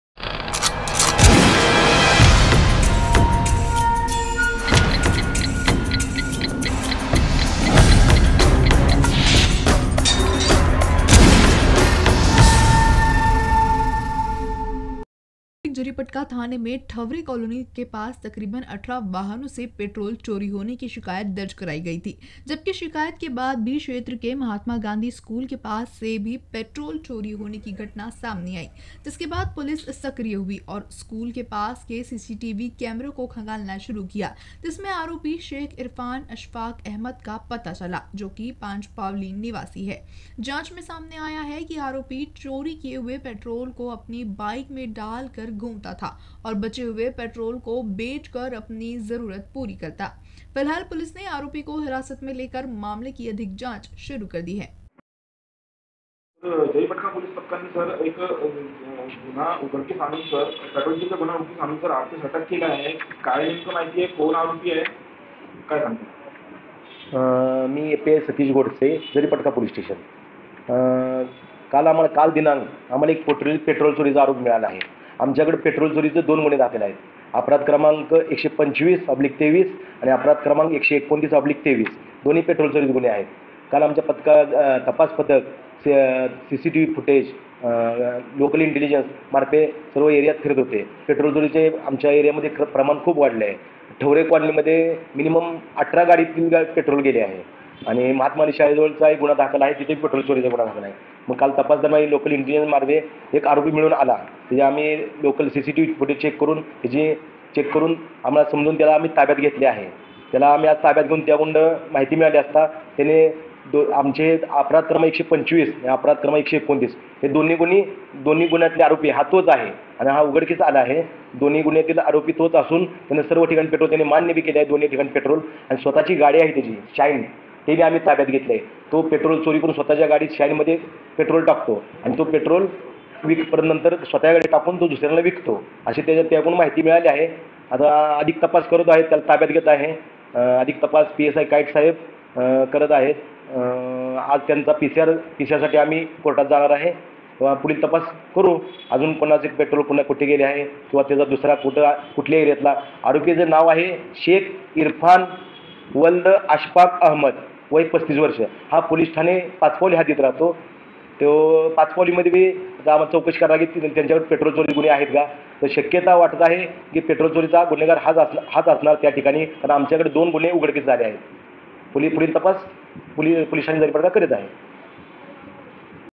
Nagpur Jaripatka | 18 वाहनों से पेट्रोल चोरी करने वाले आरोपी को किया गिरफ्तार | Nagpur Daily update
Nagpur Jaripatka | 18 वाहनों से पेट्रोल चोरी करने वाले आरोपी को किया गिरफ्तार

LIKE ➡ SHARE ➡  For latest update
@Nagpur Daily Update